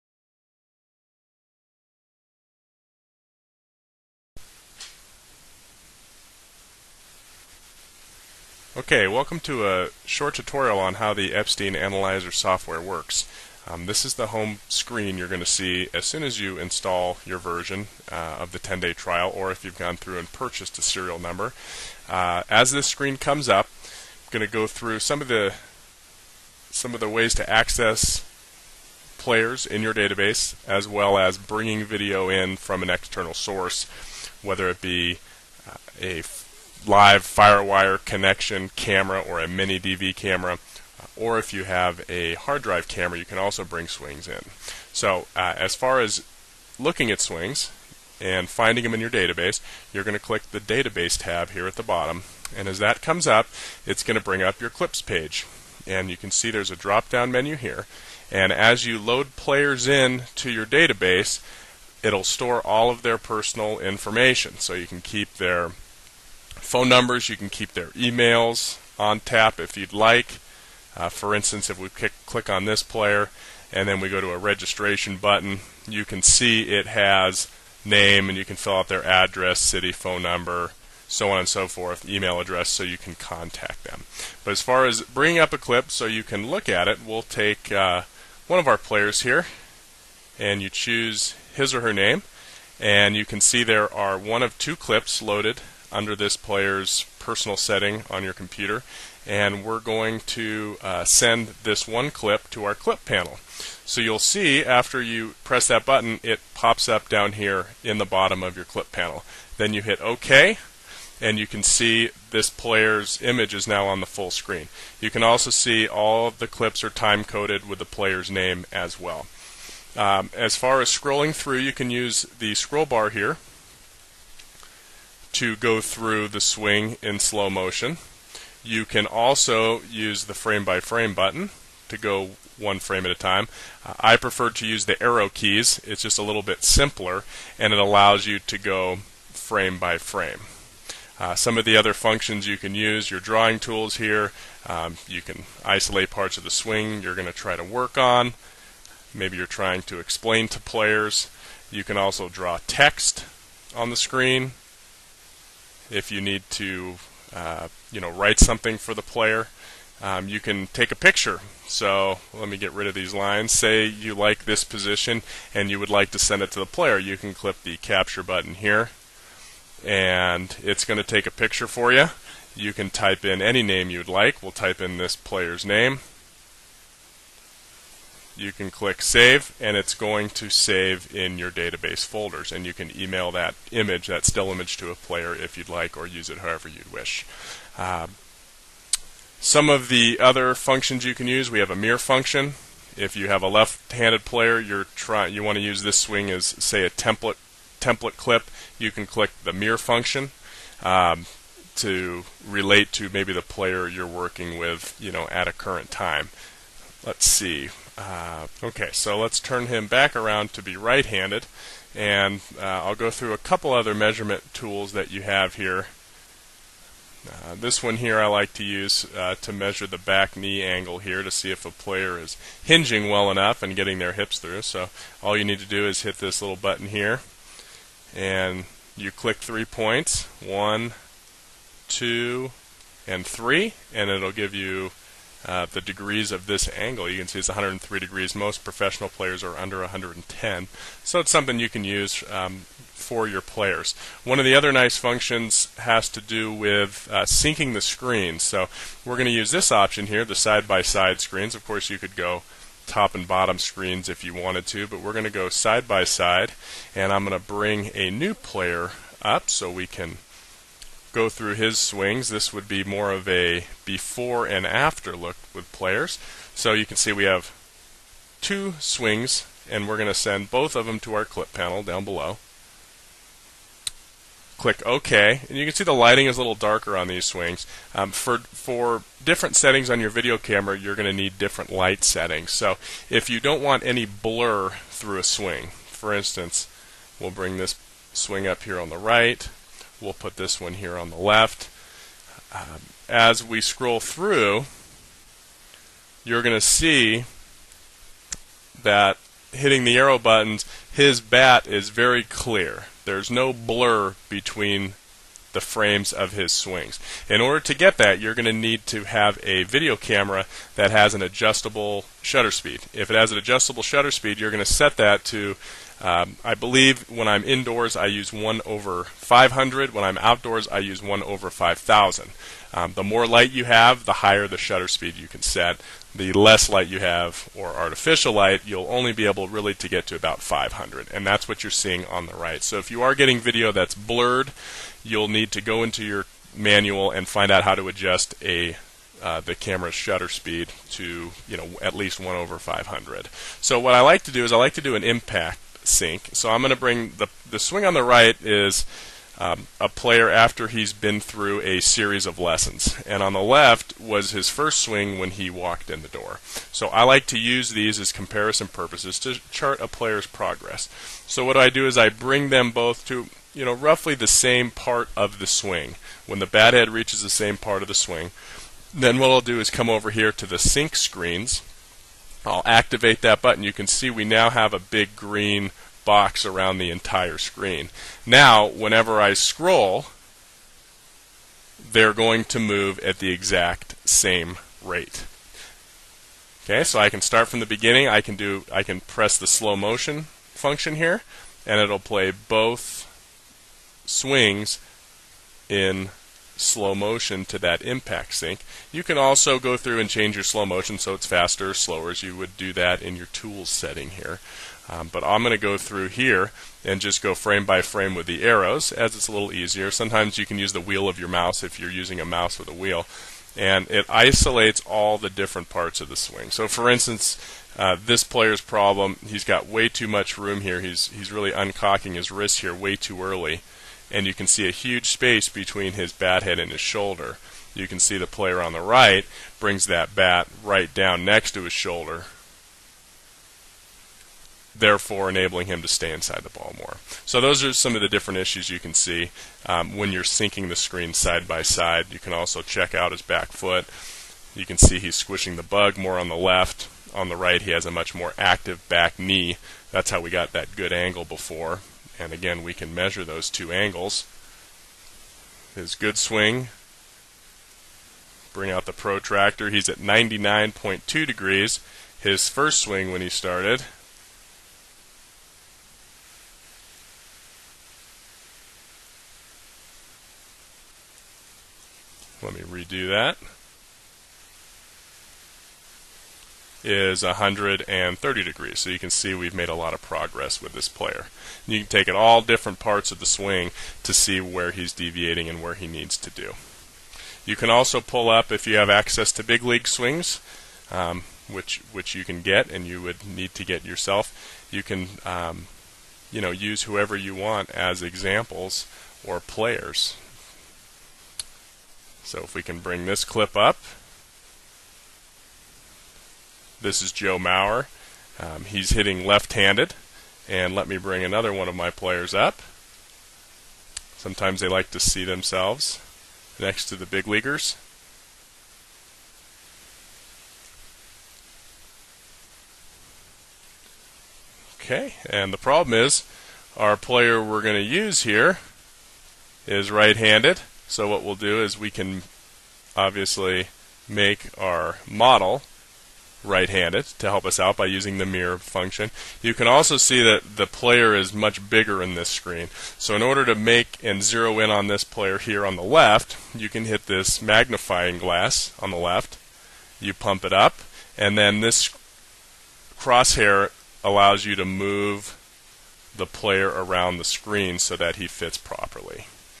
Swing Analysis Software - Mike Epstein Hitting Part 1
By far the easiest swing anlaysis software on the market today. for your FREE 10-day trial of our swing analysis software.